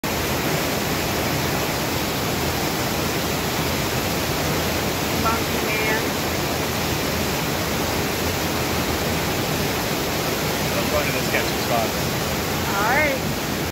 Would You Climb This For A Picture?
We went on a trip to travel Vancouver Island in a Camper Van and this is one of the spots we stopped at to take a few photos and enjoy the sounds of the waterfalls / rapids. I climb sketchy spots all the time and this one was pretty easy.